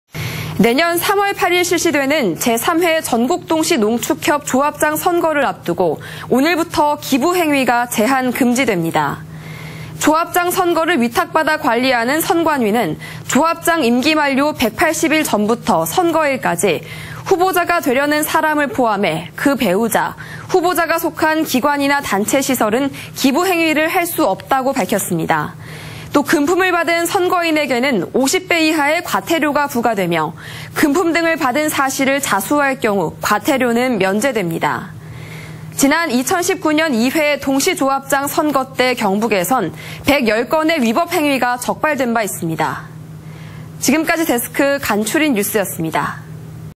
동시 조합장선거 기부행위 제한·금지 / 안동MBC
2022/09/21 10:06:53    작성자 : 김건엽

내년 3월 8일 실시되는
제3회 전국 동시 농축협 조합장선거를 앞두고
오늘(21일)부터 기부행위가 제한·금지됩니다.
◀ＥＮＤ▶

조합장 선거를 위탁받아 관리하는 선관위는
조합장 임기만료 180일전부터 선거일까지
후보자가 되려는 사람을 포함해
그 배우자, 후보자가 속한 기관이나 단체·
시설은 기부행위를 할 수 없다고 밝혔습니다.

또 금품을 받은 선거인에게는 50배 이하의
과태료가 부과되며 금품 등을 받은 사실을
자수할 경우 과태료는 면제됩니다.

지난 2019년 2회 동시조합장 선거때
경북에서는 110건의 위법행위가 적발돼
1회때 104건 보다 소폭 줄었지만
기부행위 고발건은 증가했습니다.